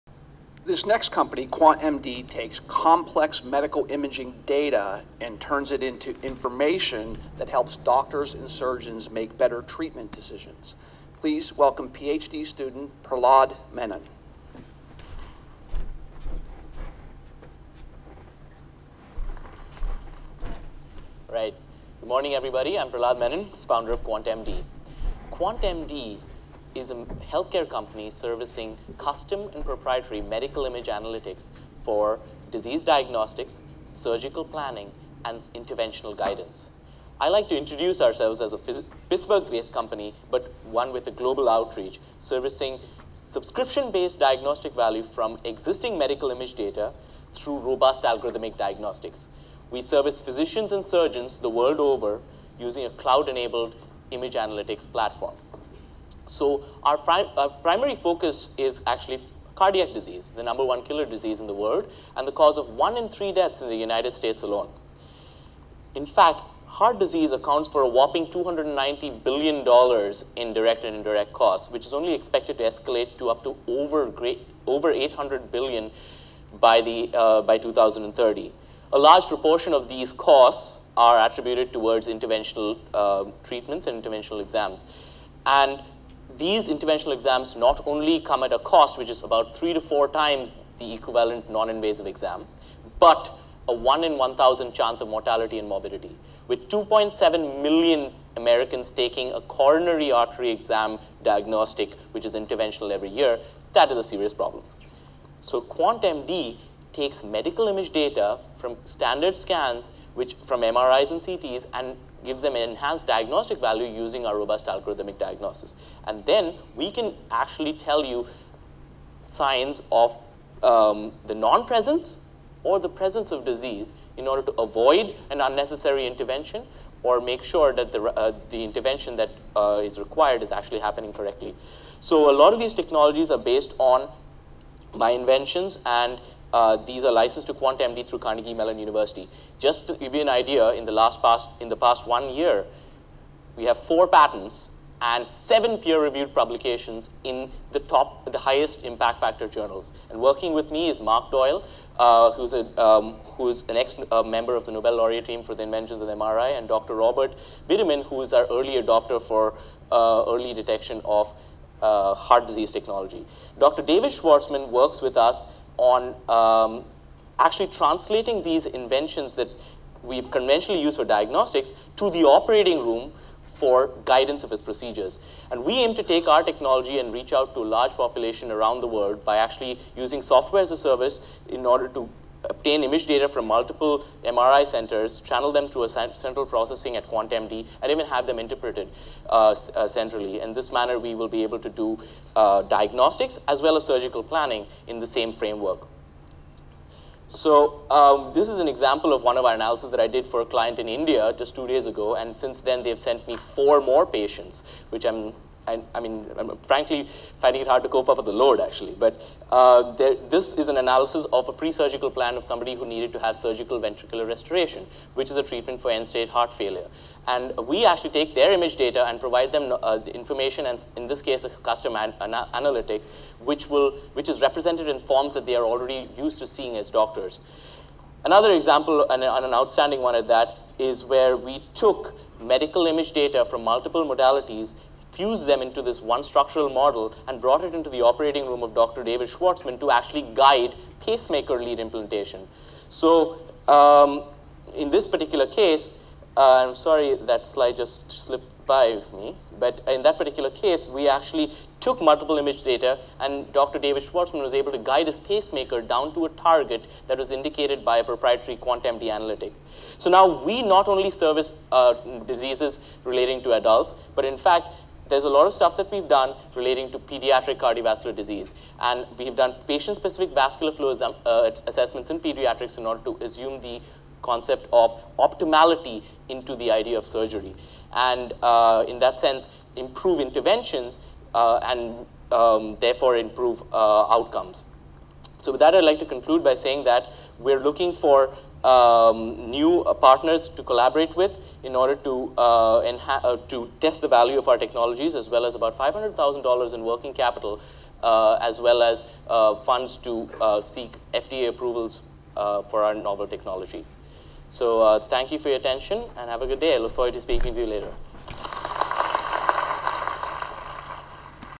QuantMD, LLC: i6 Demo Day 2012
QuantMD is a healthcare startup offering software-as-a-service custom and proprietary medical image analytics disease diagnostics, surgical planning and intra-operative guidance. We are a Pittsburgh-based company with a global outreach servicing subscription based enhanced diagnostic value from medical image data through robust algorithmic diagnostics.  We service physicians and surgeons by disambiguating patient management through our quantitative metrics offered via a cloud enabled image analytics.